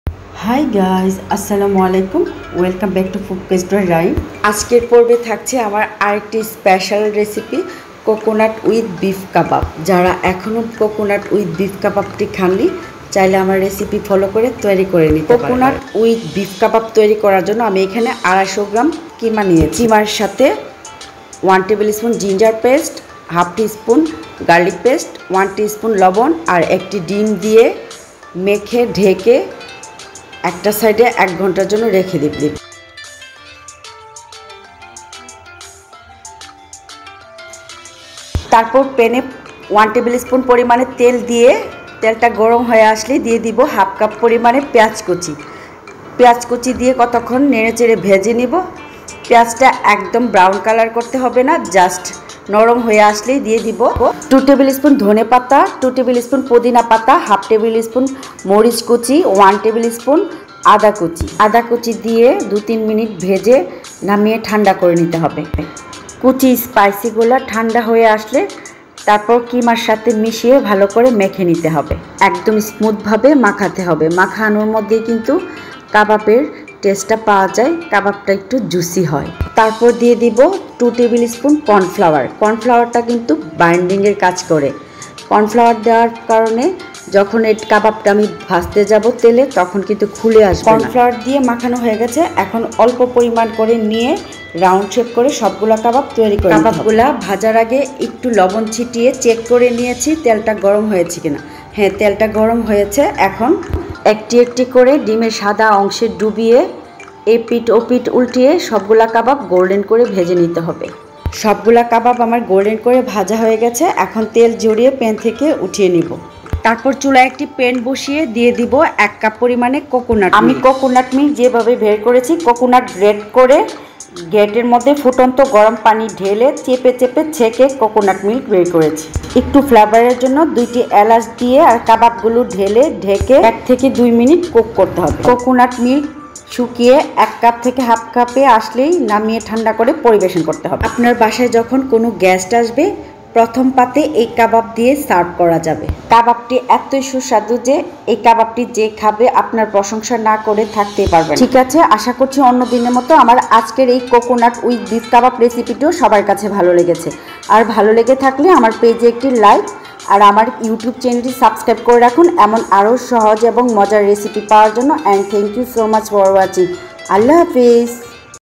( ঈদ স্পেশাল ) with beef kebab recipe | কোকোনাট উইথ বিফ কাবাব রেসিপি
coconut  with  beef  kebab recipe | কোকোনাট উইথ বিফ কাবাব রেসিপি |
উপকরণ : বিফ  কিমা ২৫০ গ্রাম
আদা ১ টেবিল চামচ।
রসুন  ১/২ টেবিল চামচ।
লবণ ১ চা চামচ।
ডিম ১।
প্রস্তুত প্রণালীঃ উপরের চারটি উপকরণ দিয়ে কিমার সাথে  ভালো করে মেখে ঢেকেএকঘন্টা মেরিনেট করে রাখতে হবে।
তারপর,  ১ টেবিল চামচ আদা কুচি।
১/২ কাপ পেঁয়াজ কুচি।
১/২ টেবিল চামচ কাঁচা মরিচ কুচি।
২  টেবিল চামচ ধনেপাতা কুচি।
২ টেবিল চামচ  পদিনা  পাতা কুচি, এ পাঁচটি উপকরণ ১ টেবিল চামচ তেল দিয়ে ভেজে নিয়ে মেরিনেট করা কিমার সাথে মিশিয়ে ভালো করে মেখে  তারপর দিয়ে দিতে হবে ২ টেবিল-চামচ  কর্নফ্লাওয়ার দিয়ে ভাল করে মেখে গোল গোল করে কাবাব তৈরি করে নিতে হবে। তারপর ডিমের সাদা অংশে চুবিয়ে ডুবো তেলে গোল্ডেন করে ভেজে নিতে হবে।তারপর অন্য একটি প্যানে ১ কাপ পরিমাণে নারিকেলের দুধ,দুটি এলাচ তারপর নারকেলের দুধে কাবাবগুলো ঢেলে ১ থেকে ২ মিনিট রান্না করে নামিয়ে ঠান্ডা করে পরিবেশন করতে হবে।
বিশেষ দ্রষ্টব্য:কুড়ানো নারকেলের সাথে এককাপ ফুটন্ত গরম পানি ঢেলে চেপে চেপে ছেঁকে নারকেলের দুধ বের করে নিতে হবে। চাইলে ব্লেন্ডার করেও নিতে পারেন।